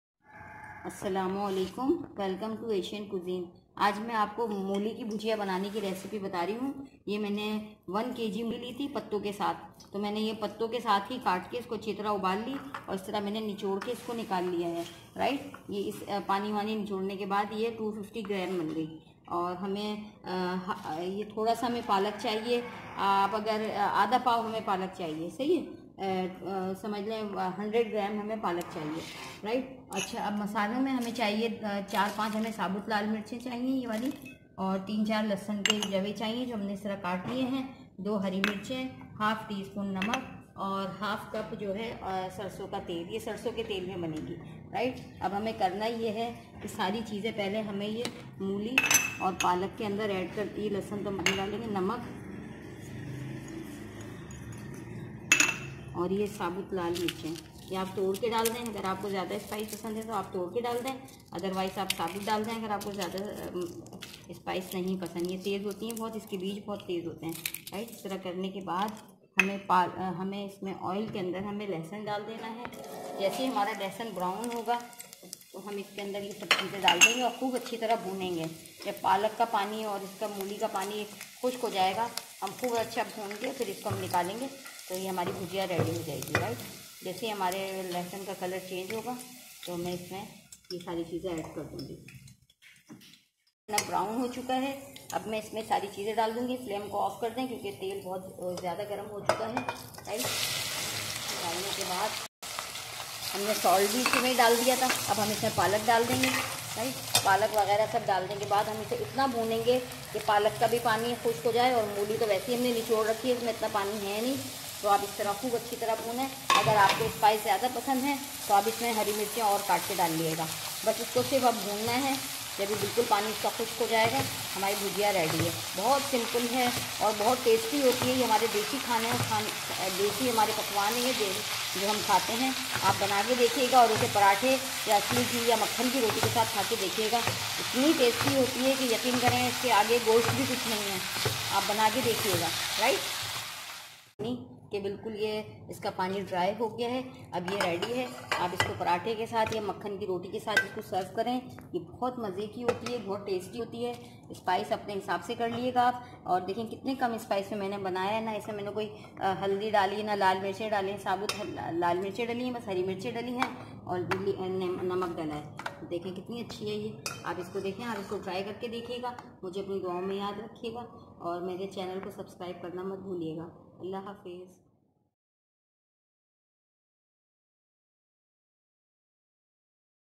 Moli Palak ki Bhujia || Easy Radish Spinach Recipe-Moli Ki Bhujia-Moli Sabzi مولی پالک کی بھجیا
Today I am going to show you how to make Moli ki Bhujia at home.
n this video I am making a quick and easy radish recipe. radish is a very healthy and nutritive treat for radish lovers. This is the season to cook radish because it has a bitter taste that almost disappears when we cook them. moli ki sabzi or bhujia is very easy and tasty to enjoy specially in winter lunch with chapatti.